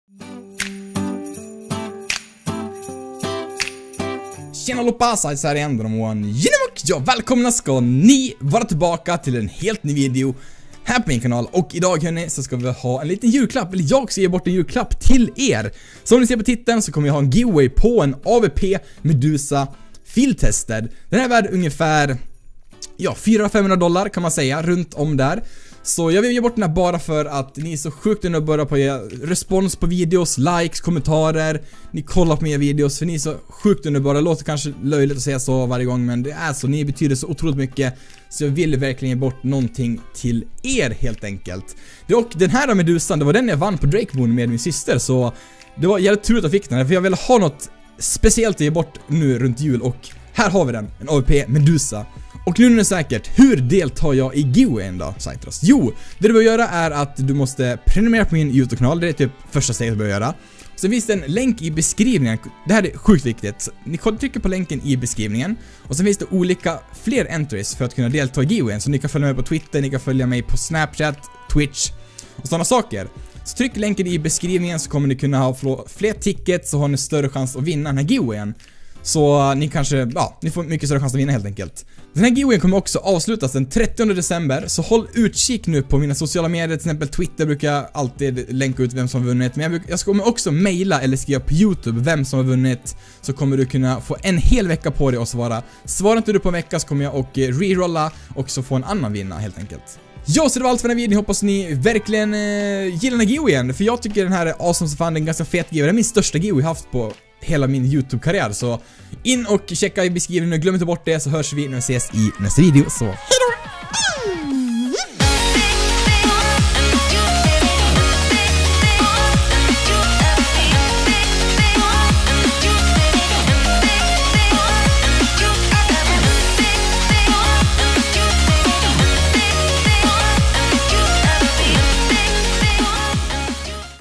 AWP MEDUSA GIVEAWAY! (AVSLUTAD!)
Här är min julklapp till er. En "AWP Medusa FT" giveaway som du har chans att vinna :D

▼Fler sidor ni kan följa mig på!▼

Tack! ♥ Whoooho!